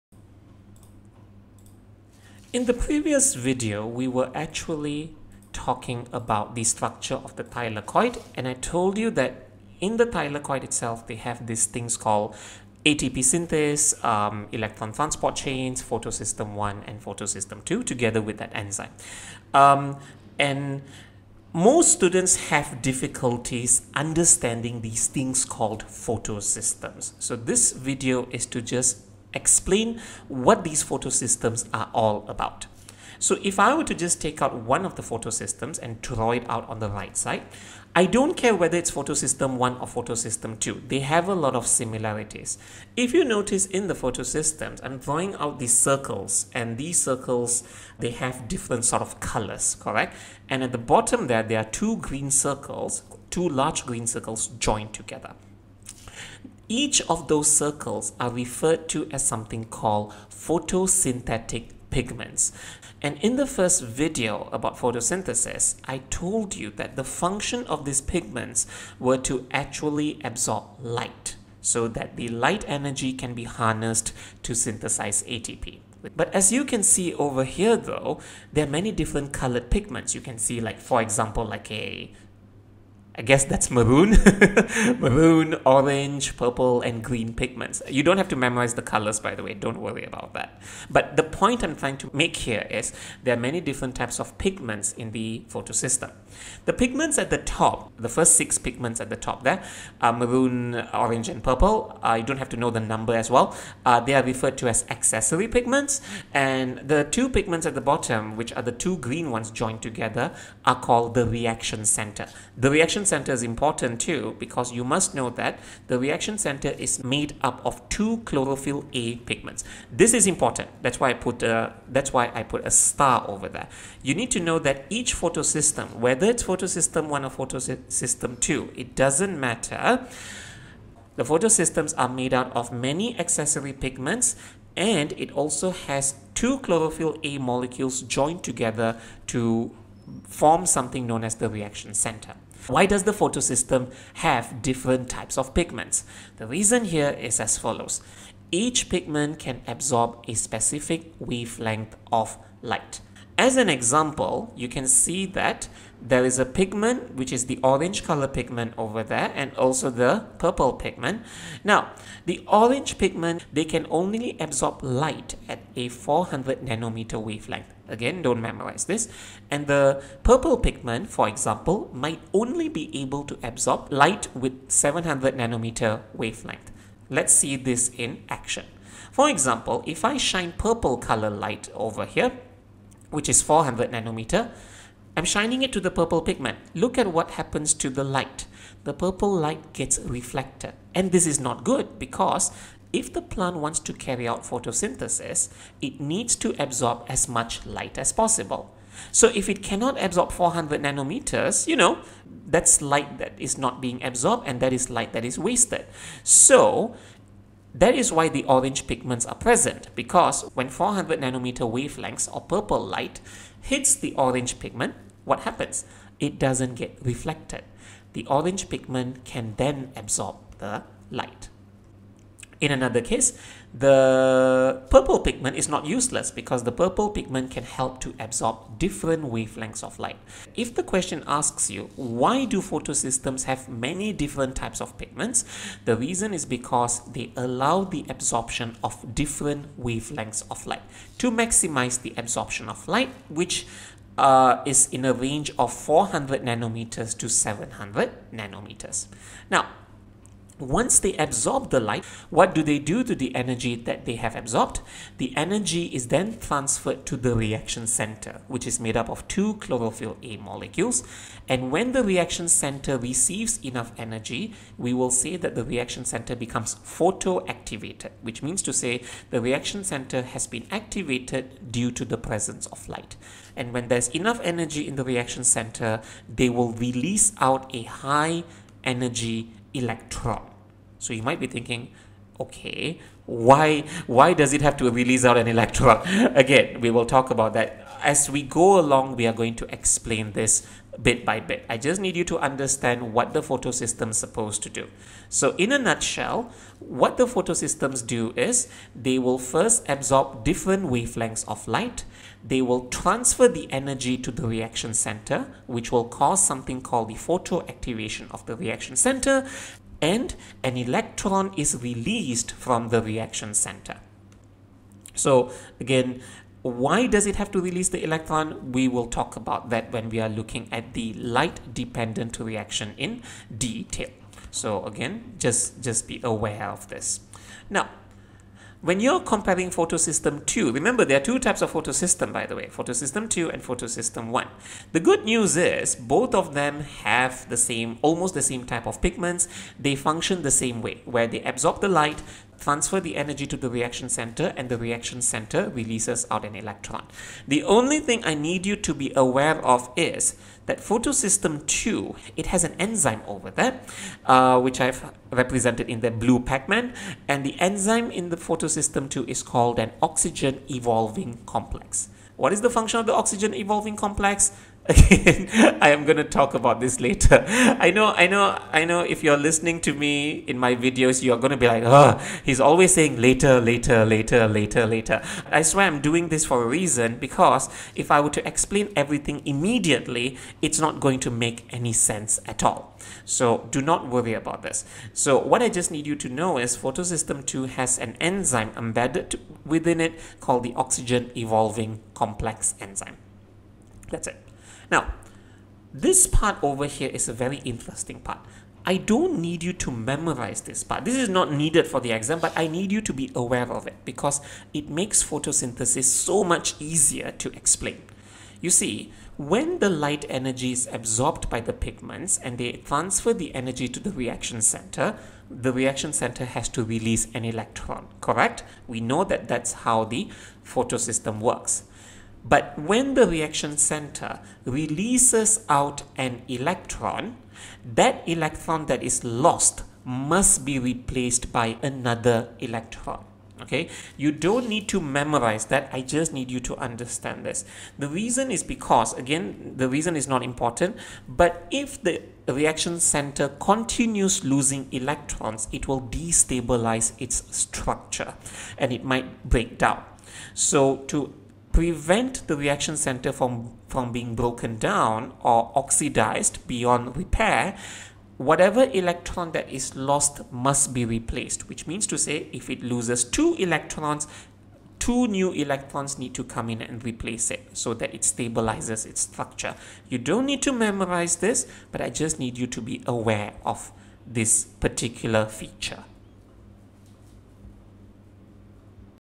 13-3 Structure and Function of the Photosystems (Cambridge AS A Level Biology, 9700)
1:18 Why does the Photosystem have many different types of pigments?
4:10 What do the Photosystems do exactly?5:30
5:30 Comparing Photosystem 1 and 2 (PS I and PS II)
6:35 (Do not memorize this part) Replacing the electron that is lost.